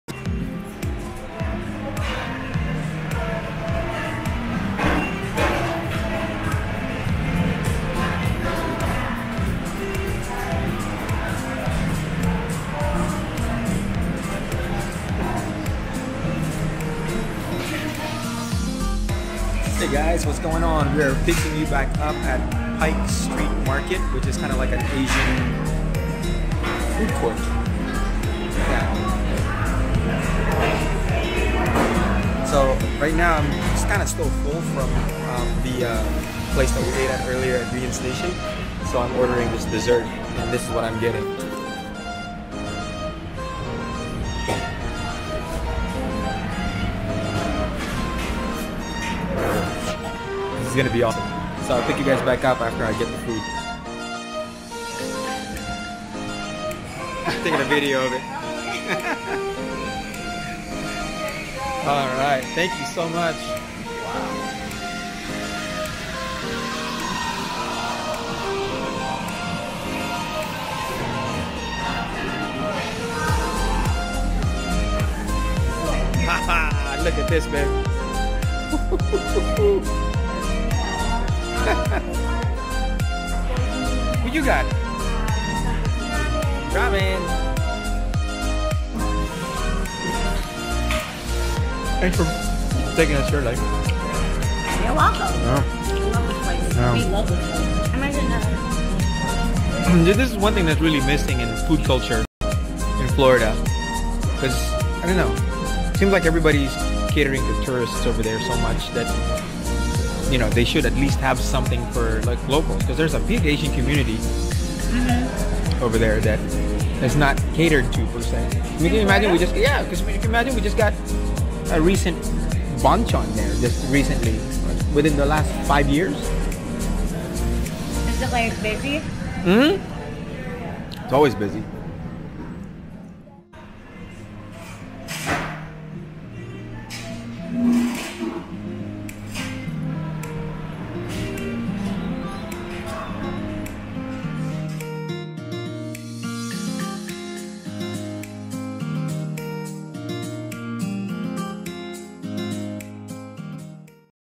Gastronomy 101 @ Pike Kitchen in Rockville, Maryland
Correction: I mentioned in my monologue that this place is "Pike Street Market" & "Food Court". It is actually called "Pike Kitchen" and the correct term is "Food Hall".

Anyway, it was fantastic! I had dessert. "Bingsoo' which is Korean flavored shaved ice with real diced mangoes, mango syrup and mango ice cream on top. I also had fried and steamed dumplings, fish cake soup and crispy ginger soy chicken. I need to come back here. and try out some of the Vietnamese fare as well.